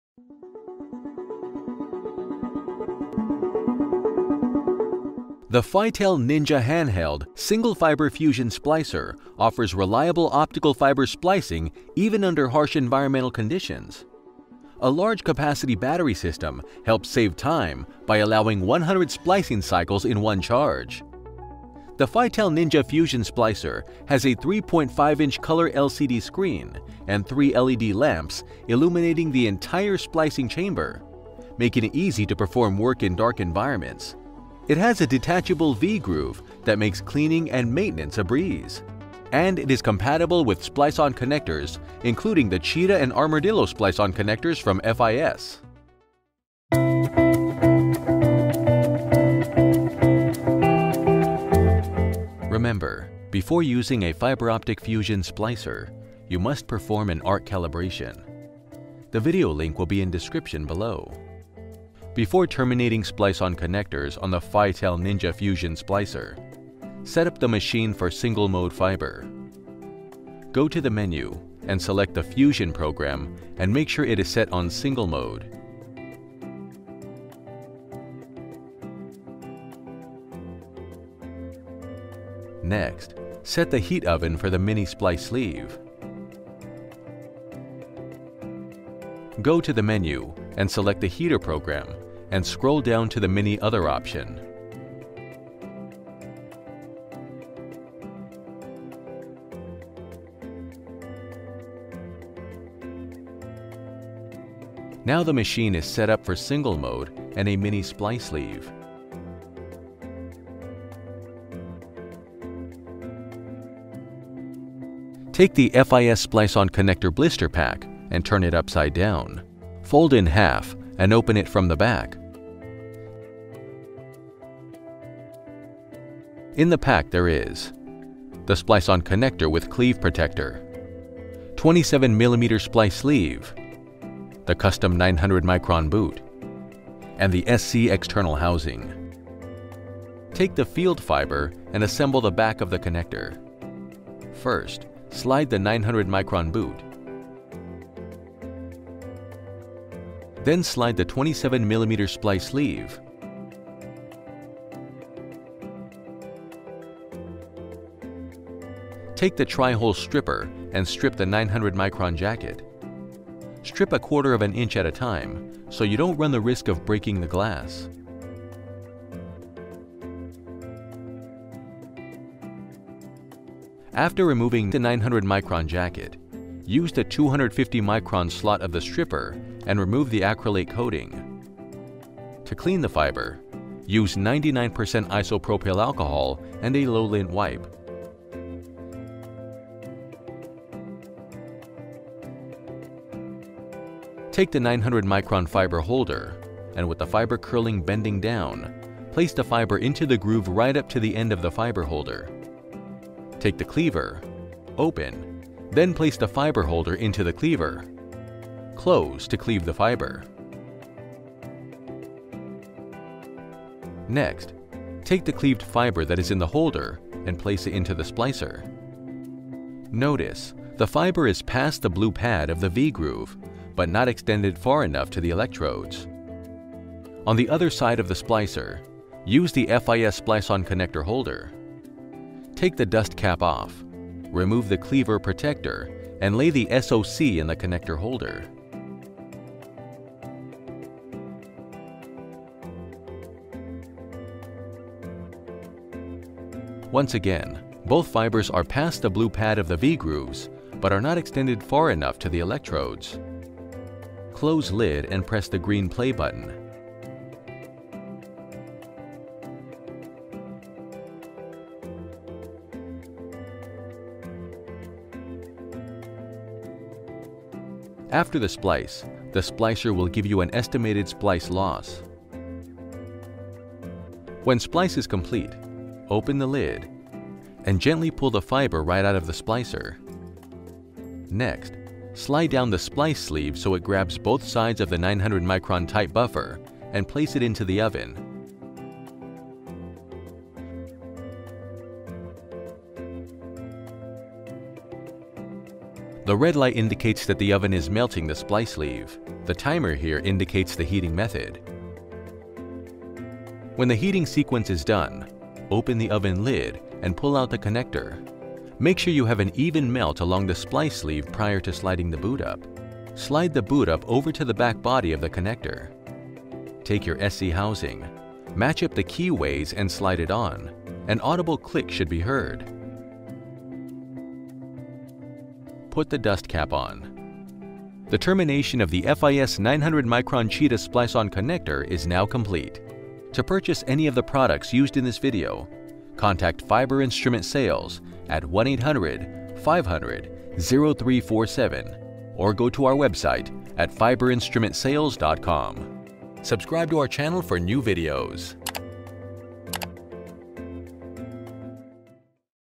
How To Terminate A 900µm Cheetah Splice On Connector On A Fitel Ninja Fusion Splicer
In this video, you will see the process for termination the FIS 900µm Cheetah Splice On Connector using Fitel Ninja Hand Held Fusion Splicer.

0:00 - Fitel Ninja Hand Held Fusion Splicer
0:47 - The 900µm Cheetah SOC Termination